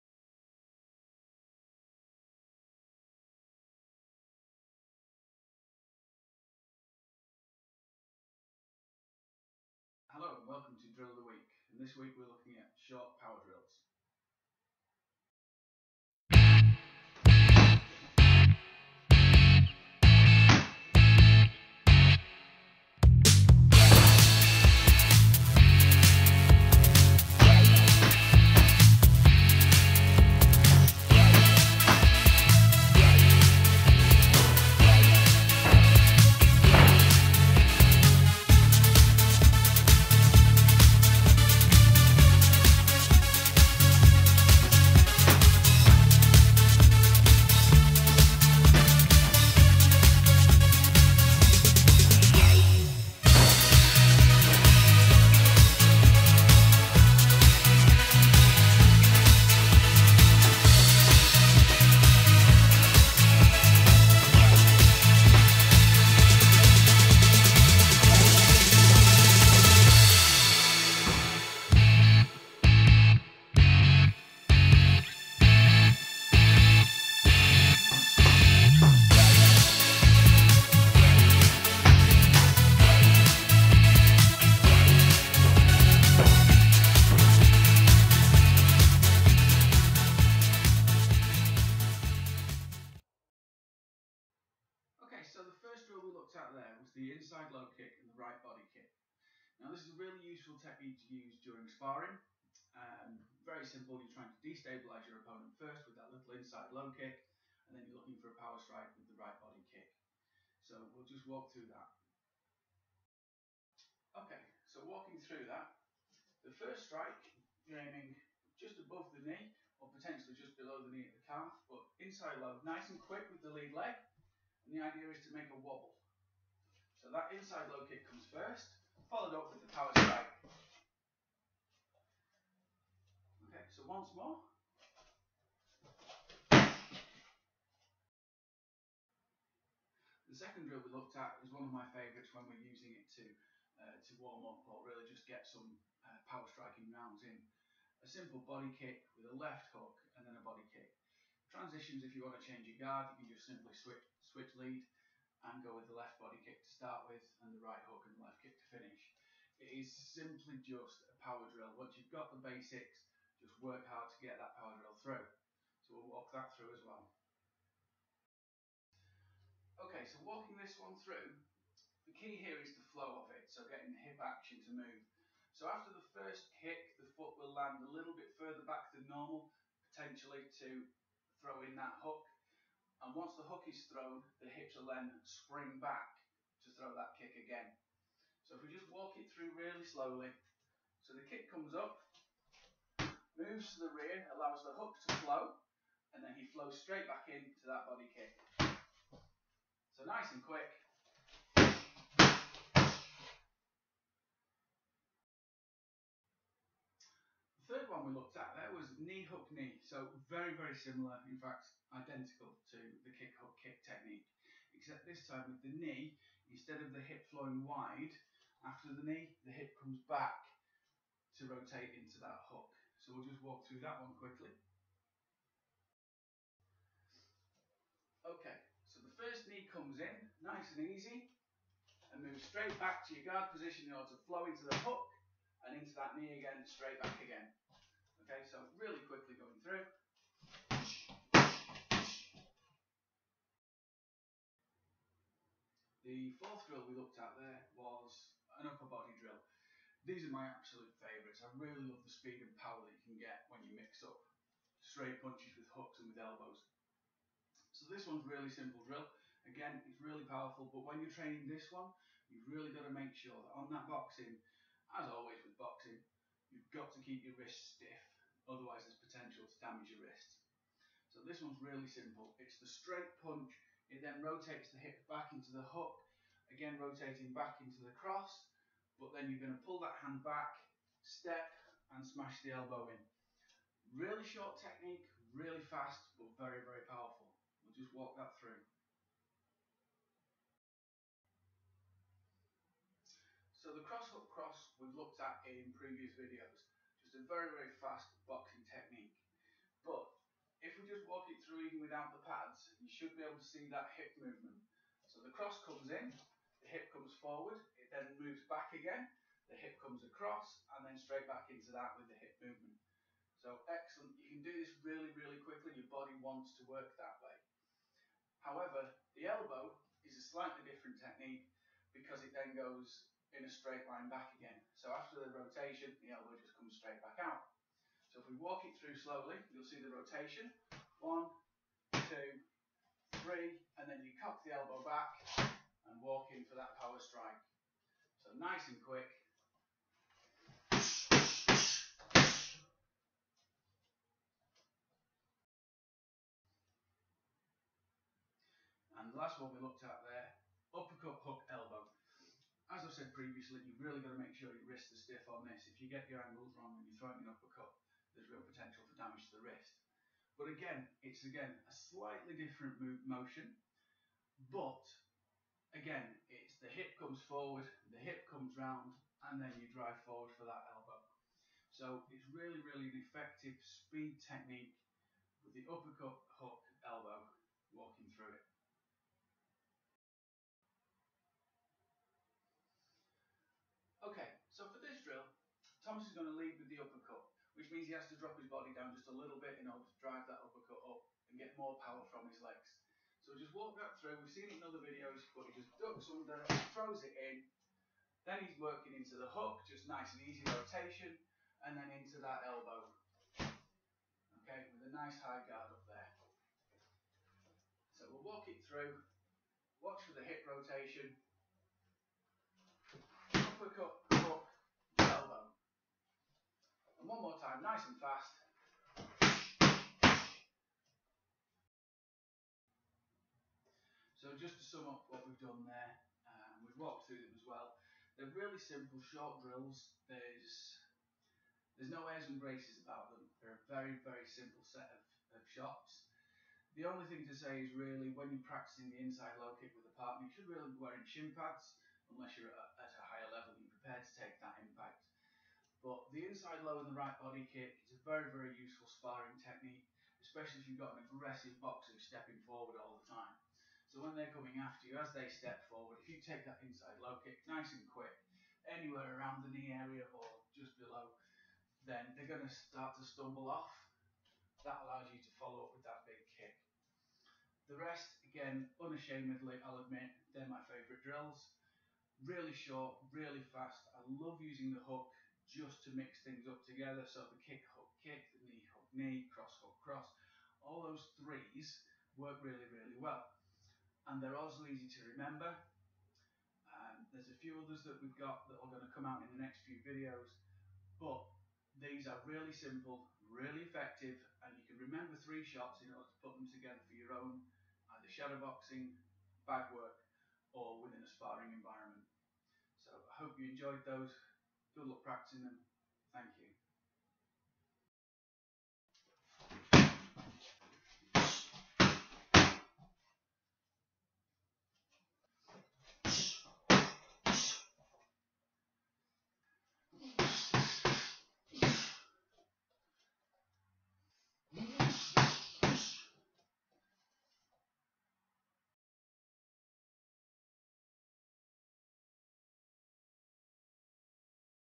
Drill Of the week - Short Power Drills
Inside Low Kick – Right Body Kick
Cross, Hook, Cross, Elbow
Kick, Hook, Kick
Knee, Hook, Knee
Uppercut, Hook, Elbow

Filmed During UK lockdown